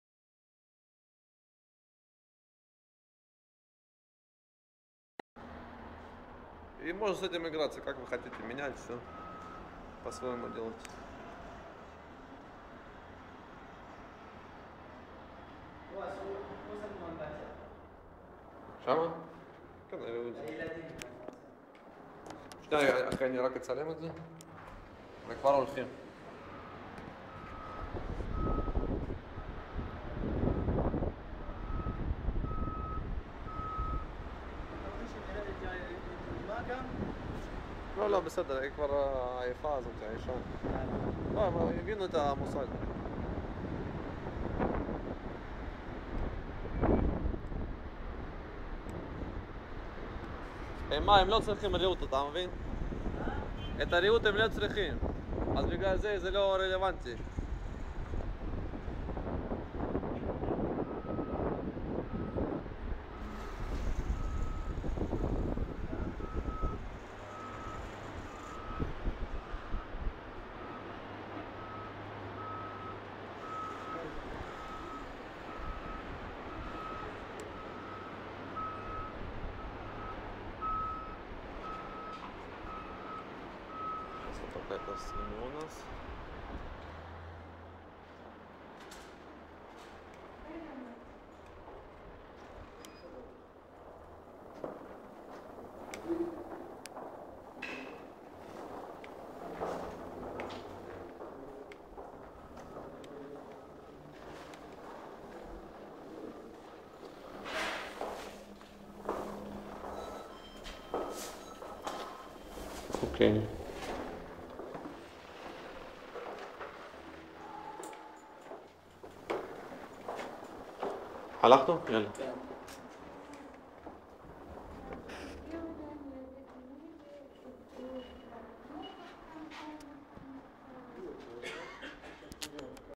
Appartement privé dans le complexe hôtel - Royal Sea
Appartement privé dans le complexe hôtel - Royal Sea. 25 étages, mangé dans un 2 appartements avec grand balcon. Le complexe dispose de: piscine, spa, salles de massage, un parking souterrain, des restaurants, magasins, salle de sport, une descente directe sur la mer.